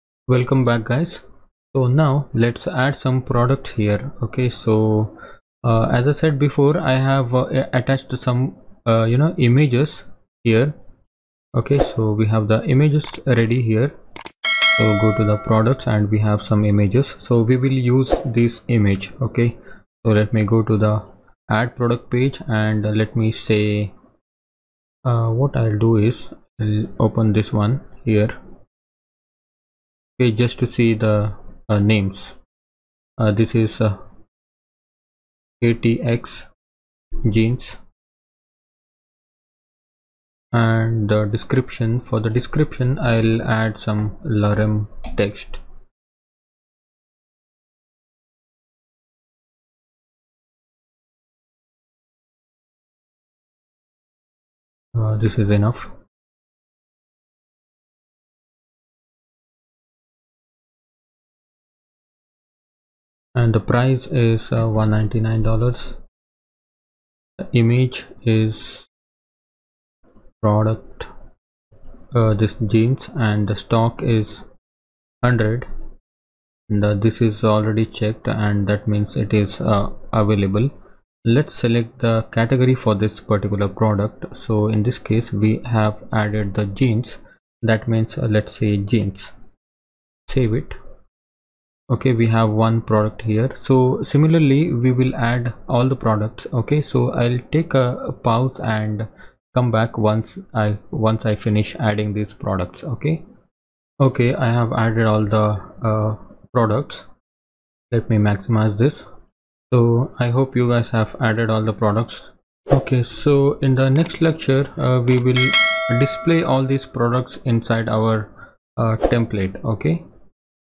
Django Ecommerce Advanced Project - 15 Add Products
Let’s start filling up your database with products! In this lesson, I’ll show you how to add products directly to the database so they’re ready to be displayed on your site. Follow along, and let’s add some items to your store!